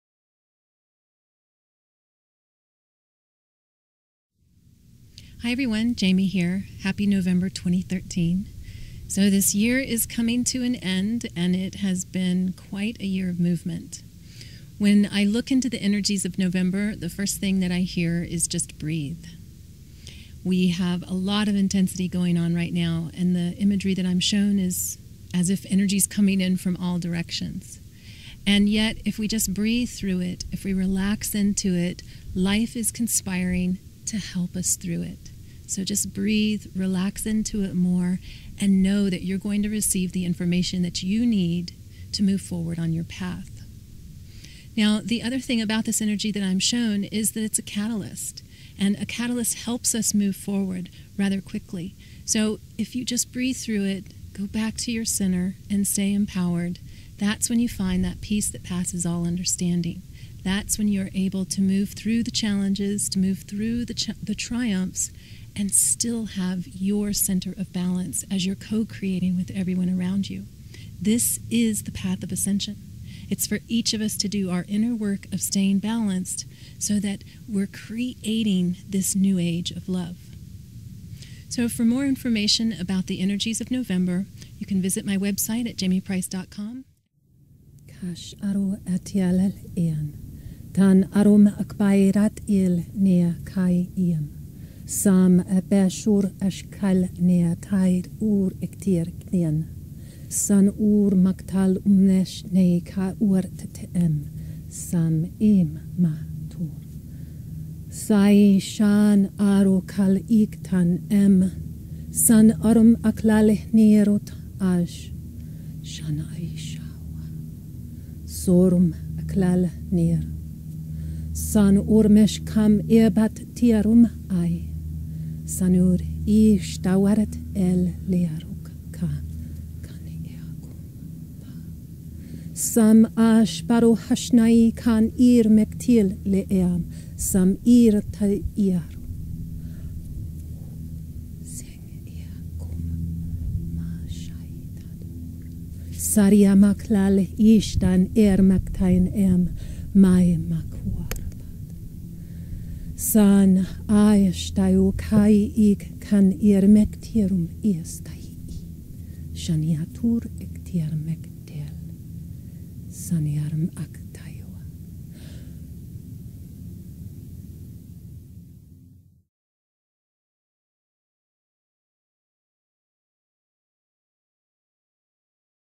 Language of Light Activation with Jamye Price - November 2013
Jamye Price is an energy healer, channel, teacher and student of life. She channels healing energies in the form of Light Language, which are ancient and universal languages.